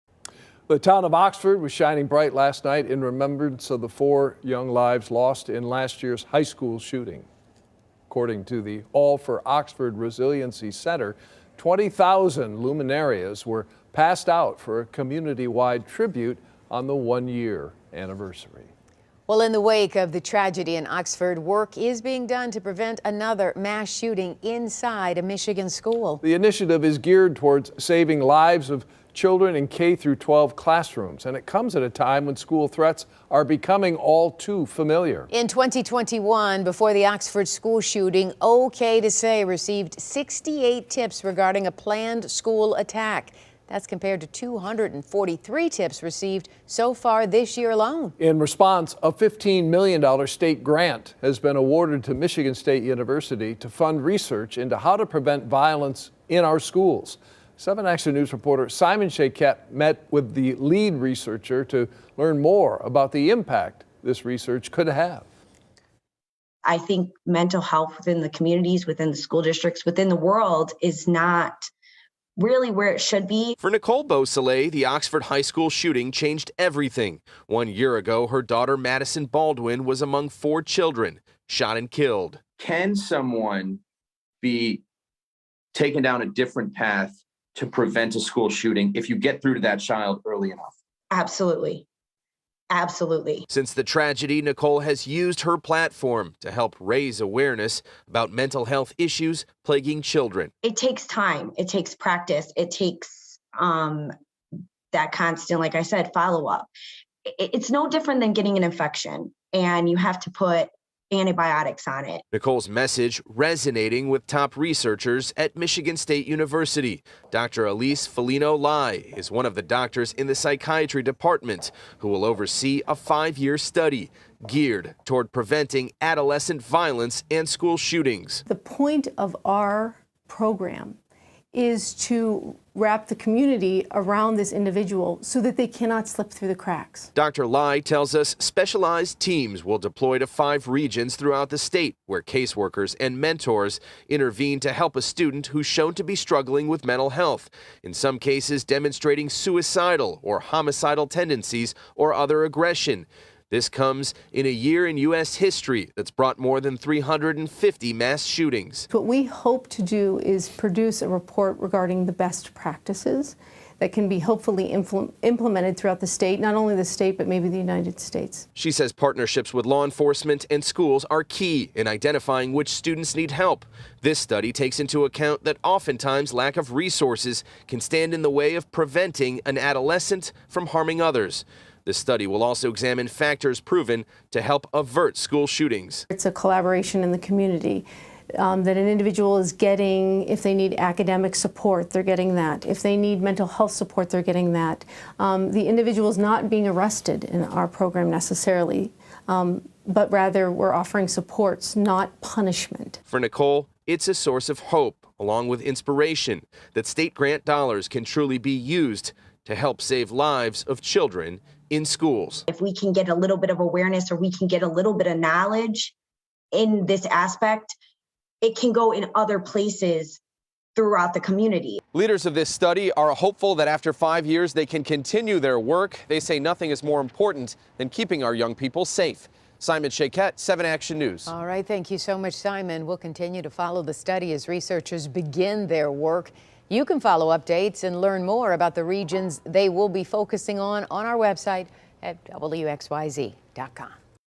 Mental health intervention is key to preventing school shootings, experts say
For Nicole Beausoleil, the Oxford High School shooting changed everything. One year ago, her daughter Madisyn Baldwin was among four children shot and killed.